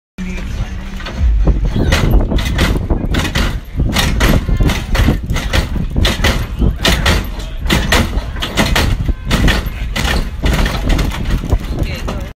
1993 Cadillac Fleetwood Brougham street car hopping
Facee Capone hitting the switch on his Cadillac.